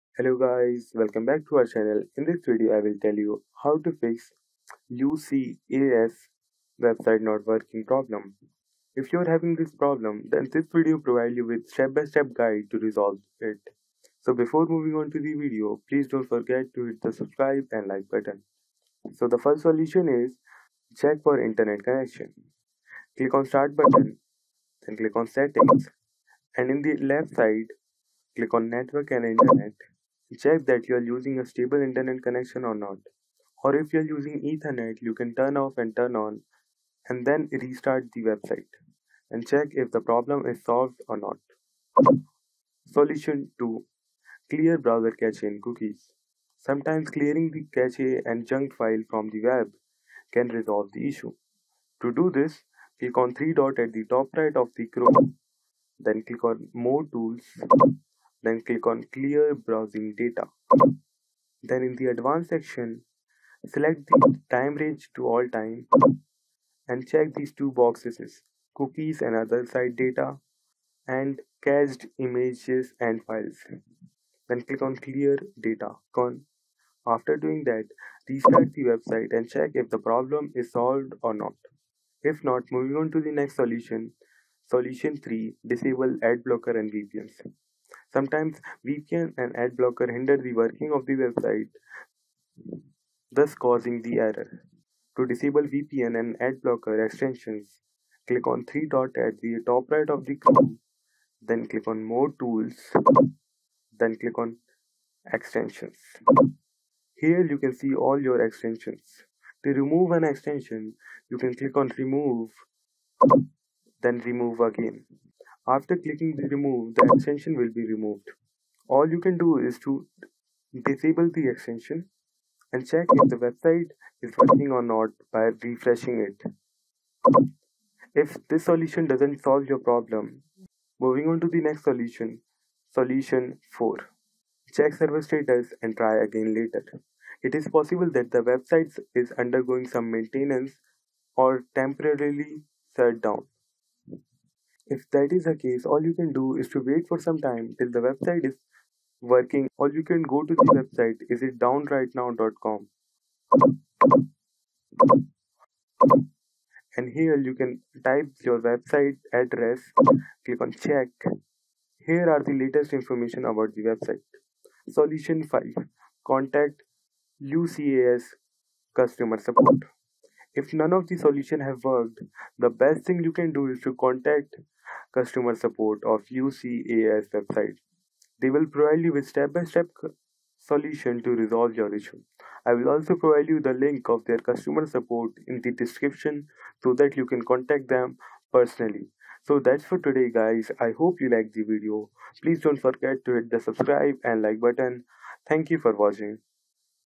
How To Fix UCAS Hub Not Working Problem
Encountering issues with the UCAS Hub? This comprehensive guide will assist you in troubleshooting and resolving the problem. Learn how to address the 'UCAS Hub not working' issue with step-by-step solutions. From checking your internet connection and clearing browser cache and cookies to disabling ad blockers and VPNs, explore solutions to overcome login difficulties. You can also check the server status and contact UCAS customer support for further assistance. Ensure a smooth UCAS Hub experience and stay on top of your educational journey! 🎓🔒

Chapters:
00:00 Introduction
00:18 Solution 1: Check Internet Connection
00:42 Solution 2: Clear Browser Cache and Cookies
01:25 Solution 3: Disable Ad Blocker and VPNs
02:12 Solution 4: Check Server Status
02:44 Solution 5: Contact UCAS Customer Support
03:08 Conclusion